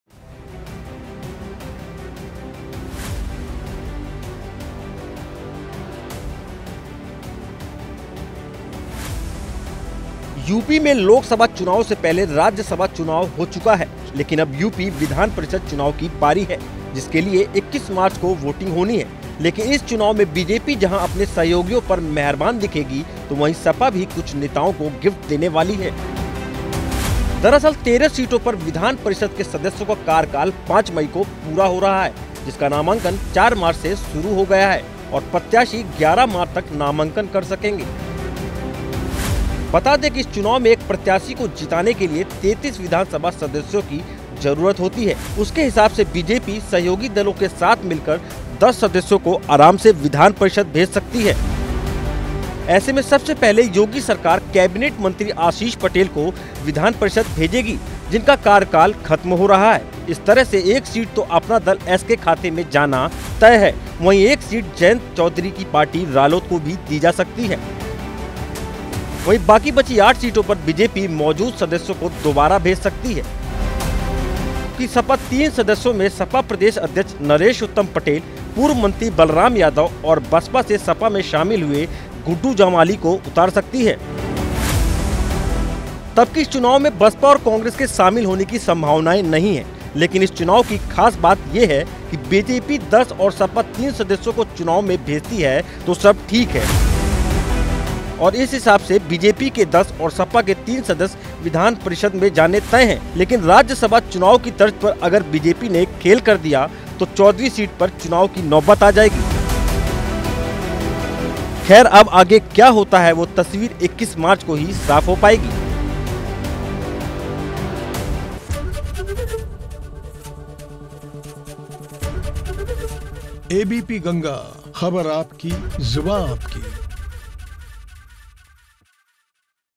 MLC Election 2024: राज्यसभा चुनाव के बाद MLC चुनाव की तैयारी,इस तारीख को होगी वोटिंग
यूपी में लोकसभा चुनाव से पहले राज्यसभा चुनाव हो चुका है, लेकिन अब यूपी विधानपरिषद चुनाव की बारी है, जिसके लिए 21 मार्च को वोटिंग होनी है, लेकिन इस चुनाव में बीजेपी जहां अपने सहयोगियों पर मेहरबान दिखेगी, तो वहीं सपा भी कुछ नेताओं को गिफ्ट देने वाली है